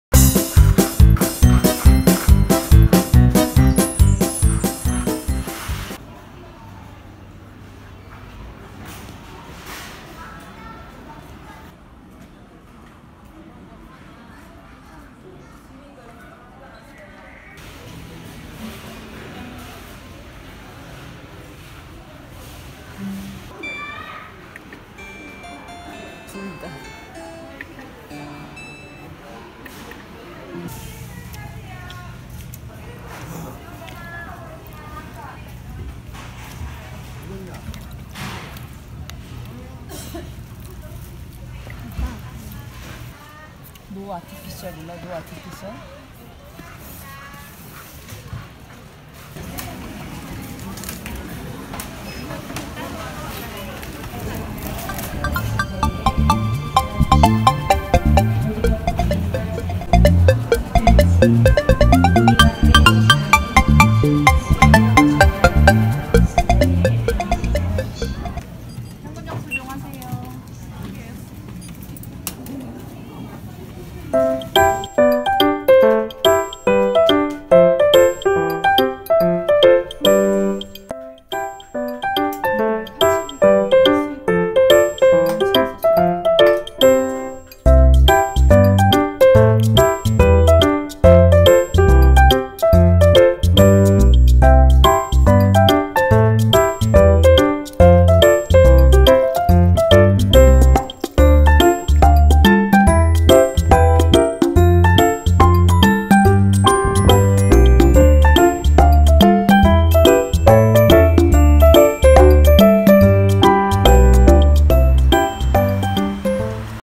마트 죽순이 이마트 코스트코 다녀옴, 시식 먹순이 기록
안녕하세요~
마트 돌아 다닐때 제일 행복해요:)
탕진잼의 끝은?!